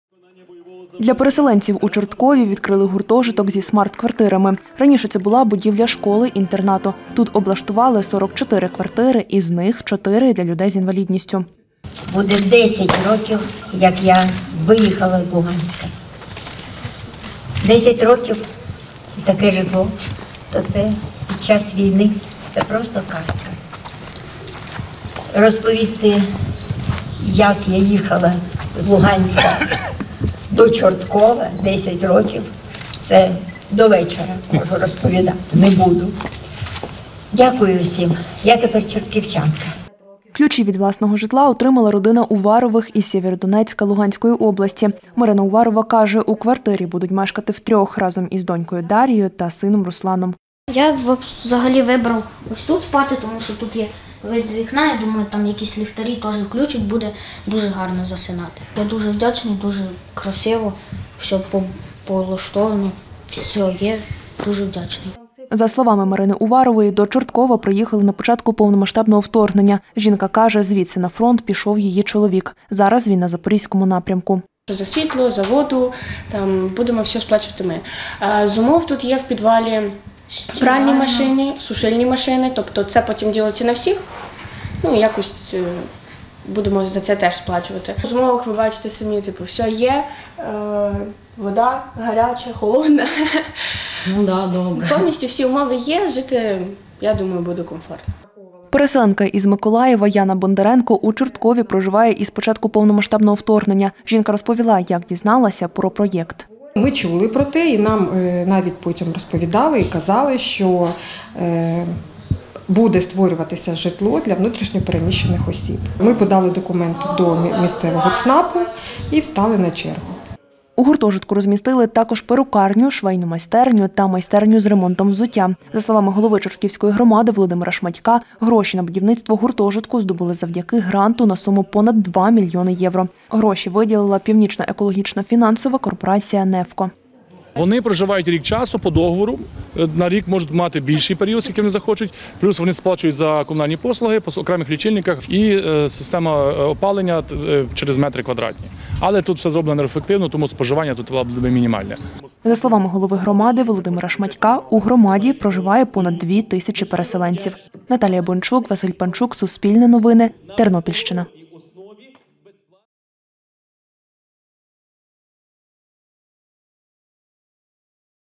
У Чорткові облаштували смарт-квартири для переселенців
Для переселенців у Чорткові на Тернопільщині відкрили гуртожиток зі смарт-квартирами. Ключі отримали 40 сімей, про це Суспільному розповів голова Чортківської громади Володимир Шматько. За його словами, проєкт реалізували завдяки гранту на суму понад два мільйони євро. Які облаштували умови та хто може поселитися у квартирах – дивіться у сюжеті Наталії Бончук.